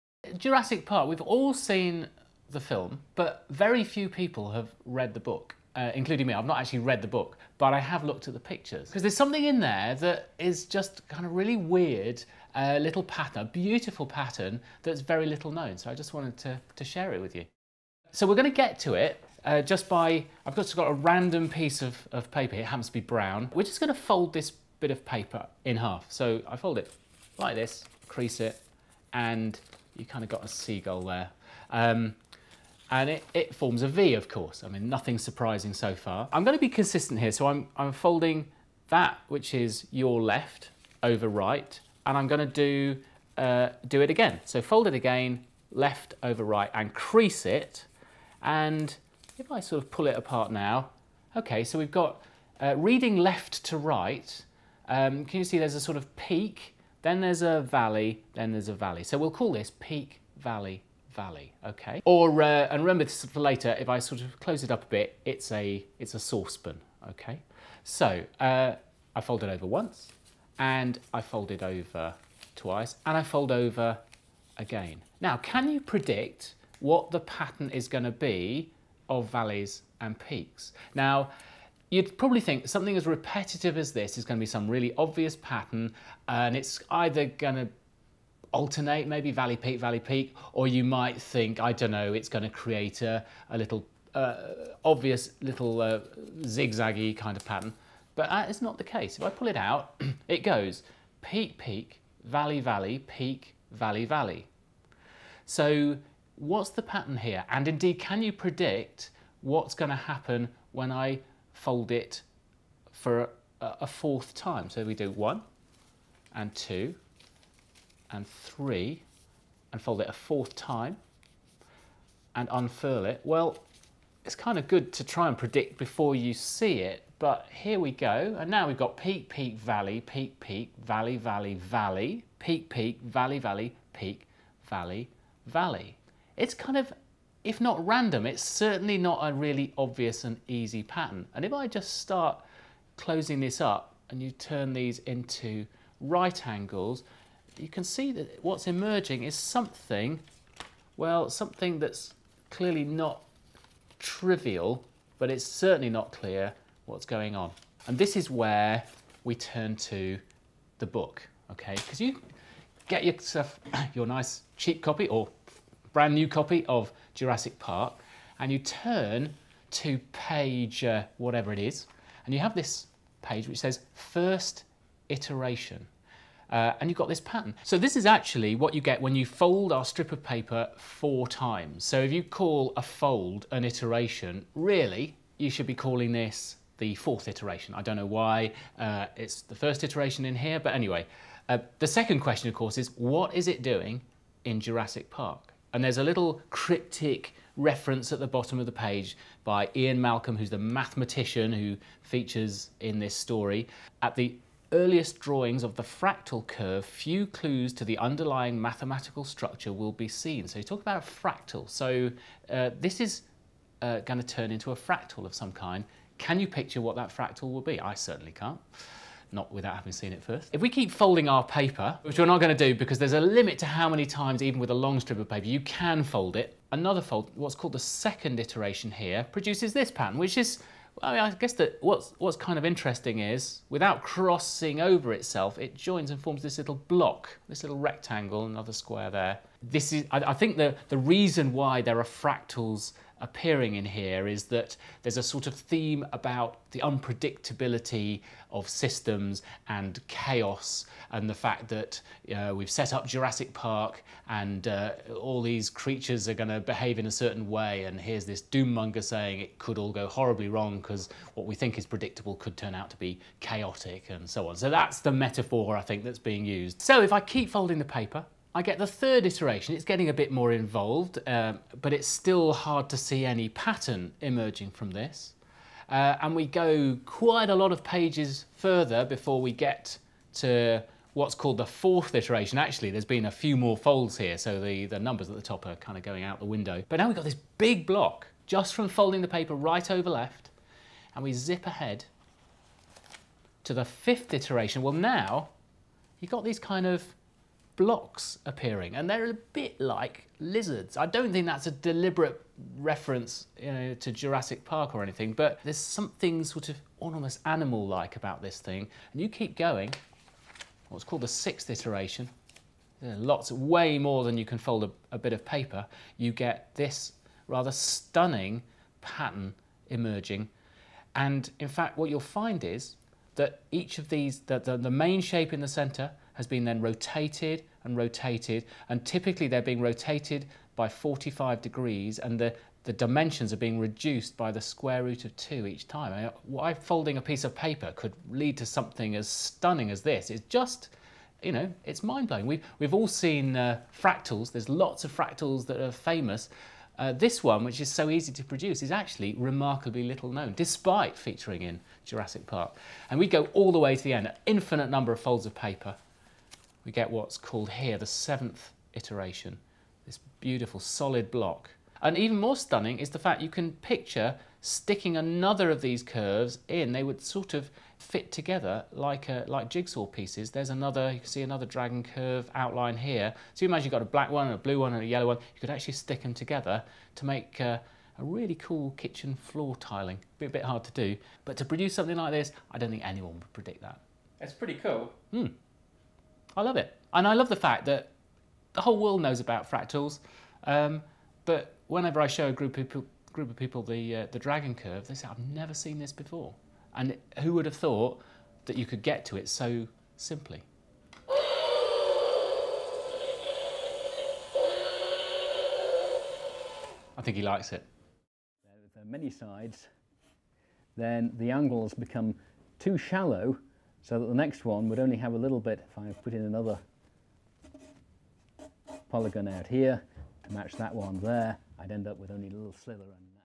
Dragon Curve - Numberphile
Beautiful Dragon Curves, Fractals and Jurassic Park. Featuring Rob Eastaway.

Thanks to Matthew Ward and Faraz Barzideh who helped Brady out with some curves!

The book Jurassic Park is by the late Michael Crichton.